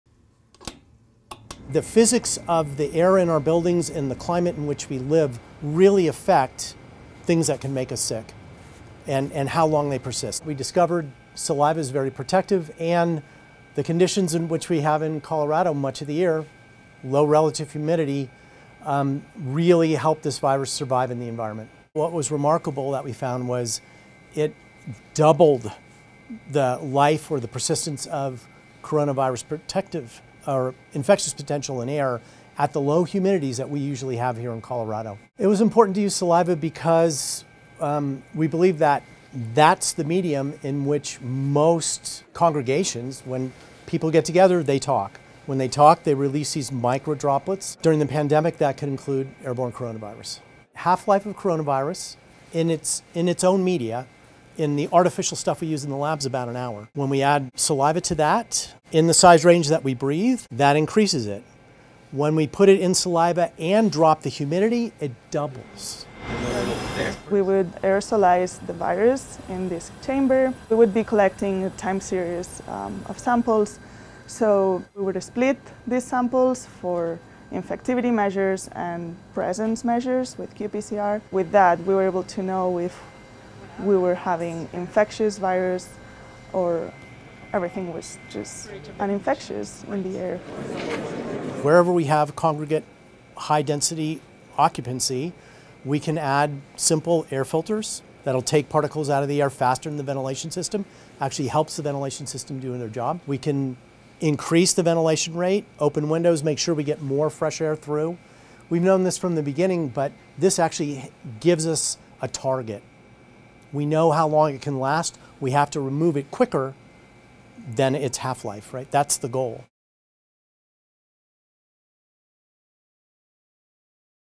Thanks to saliva, infectious coronavirus particles linger twice as long in drier air
Recent research from the University of Colorado Boulder may have finally revealed why humans tend to get sick from airborne viral diseases more often in drier environments. The study found that airborne particles carrying a mammalian coronavirus closely related to the virus which causes COVID-19 remain infectious for twice as long in drier air. This is in part because the saliva emitted with them—stabilized by sugary carbohydrates—serves as a protective barrier around the virus, especially at low humidity levels.